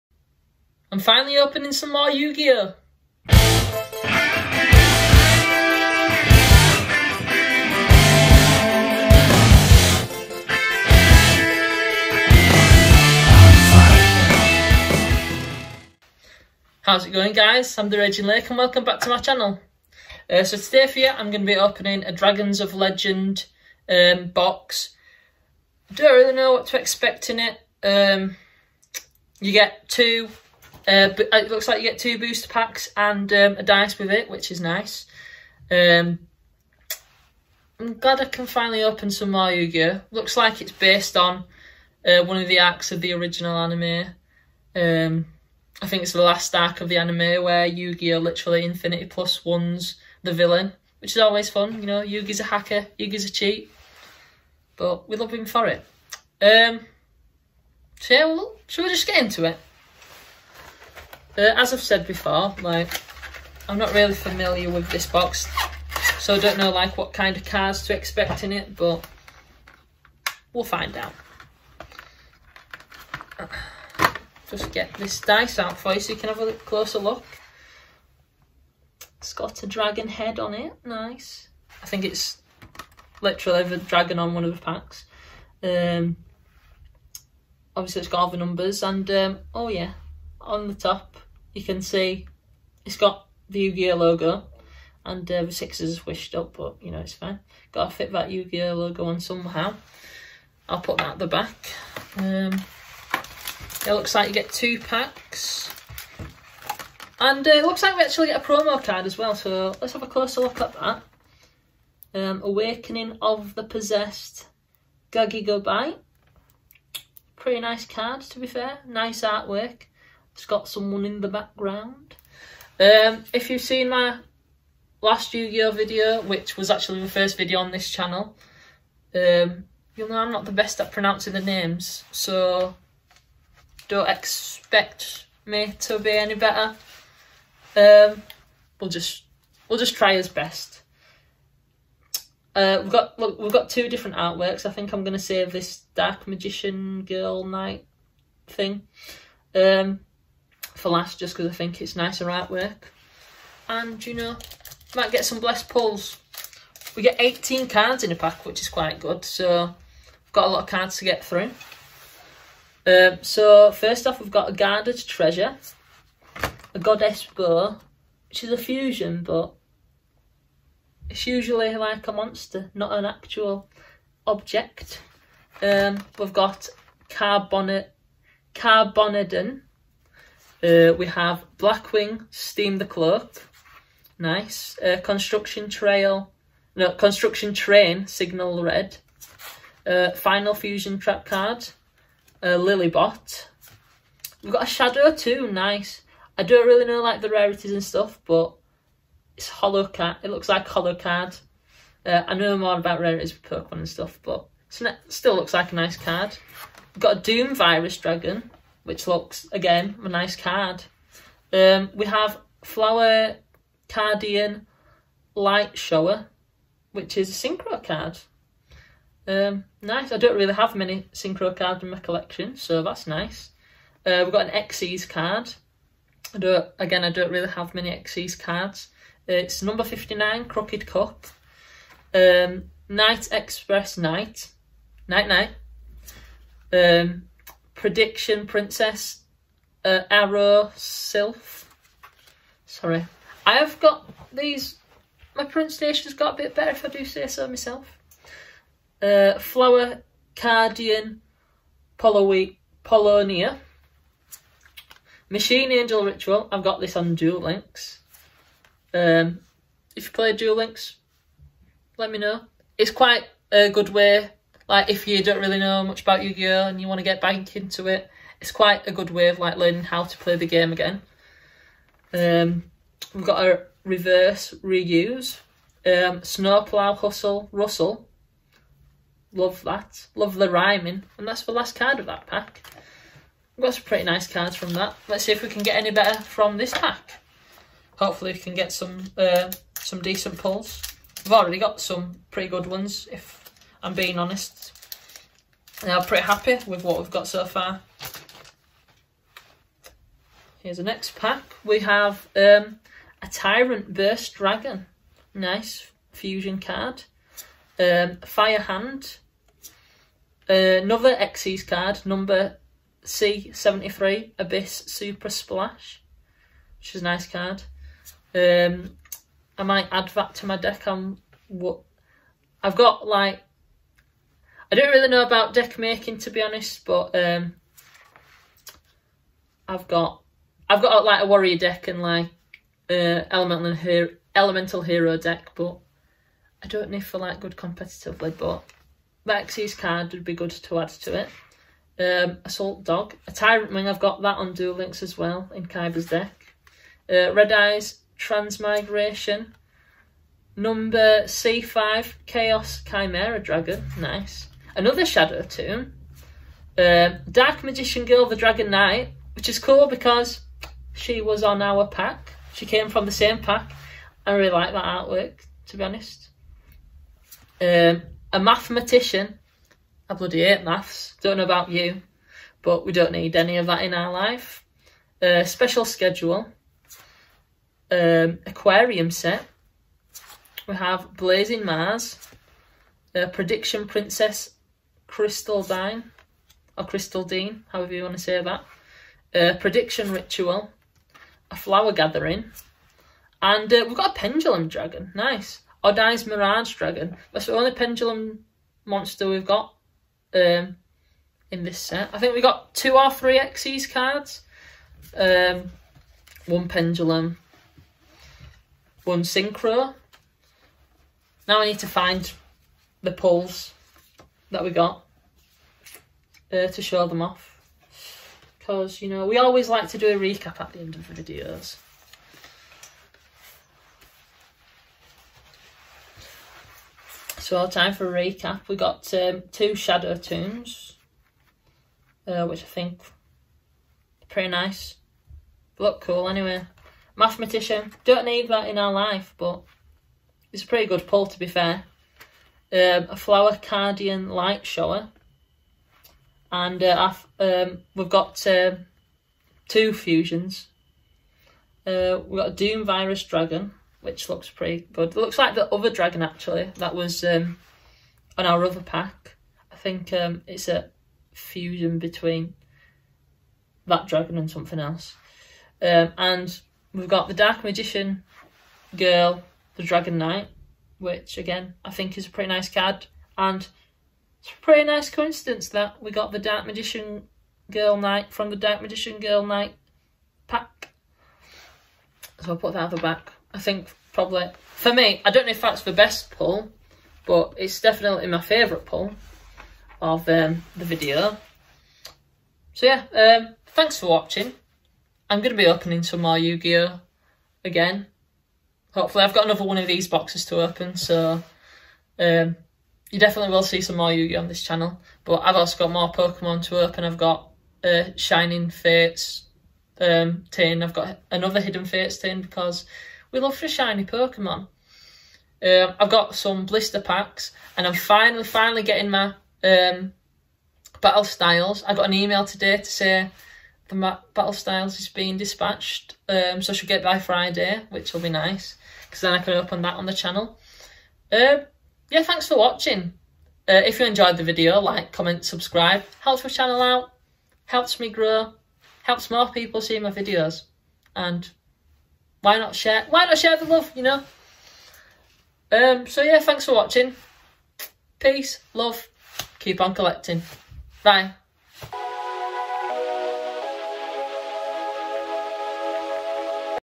Dragons of Legend Box Opening
Finally opening some more Yu-Gi-Oh! Opening a Dragons of Legend box, can we pull something good???